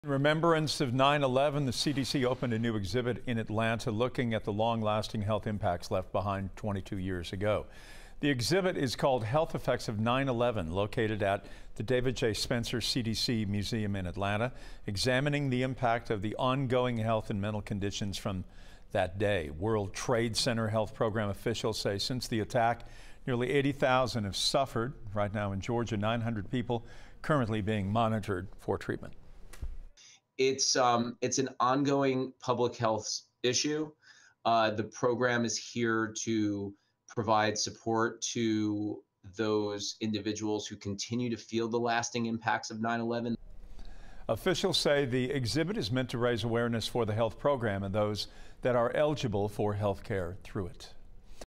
CDC new exhibit examines health impacts from 9/11
In remembrance of 9/11, the Centers for Disease Control and Prevention opened a new exhibit in Atlanta-- looking at the health impacts left behind 22 years ago.